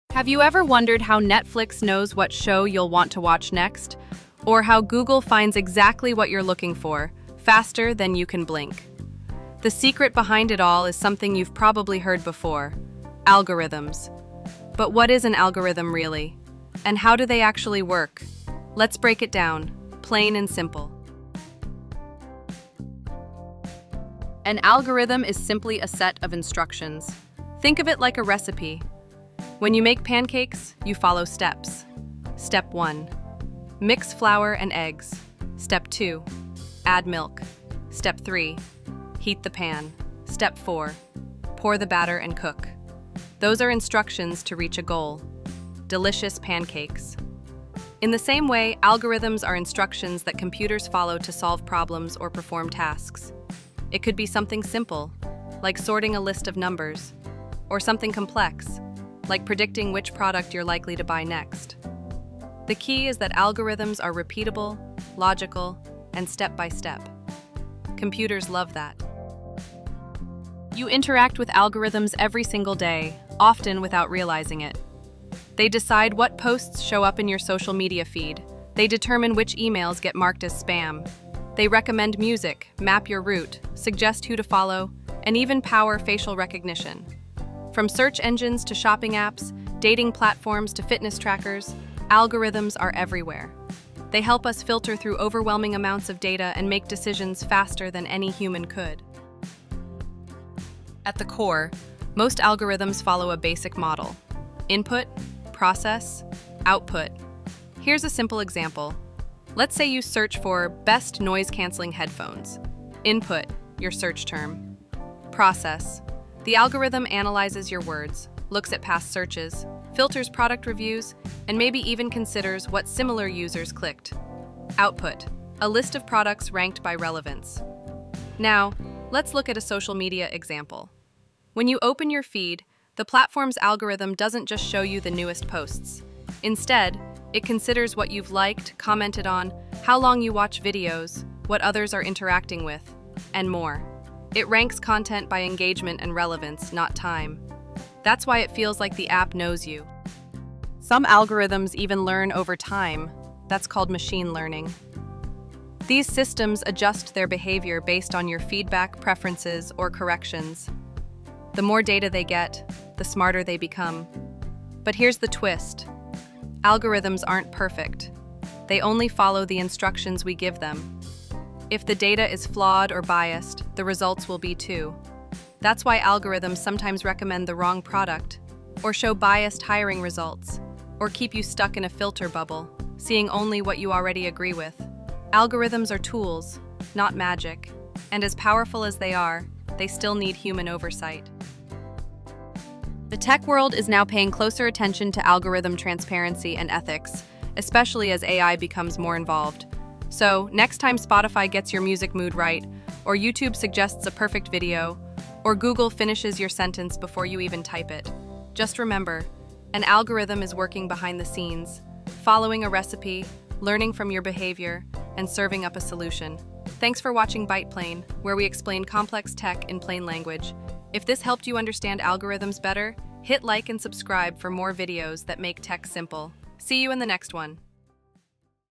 How Do Algorithms Actually Work? – Explained Simply
Algorithms power the internet — from search engines and social media to online shopping and GPS. But what is an algorithm, really?

In this BytePlain explainer, we break down algorithms in the simplest way possible. You’ll learn what they are, how they work, and why they’re so important in our digital lives. No complex code or jargon — just a clear, visual explanation.

If you've ever wondered how your apps know what to recommend or how Google sorts results, this video is for you.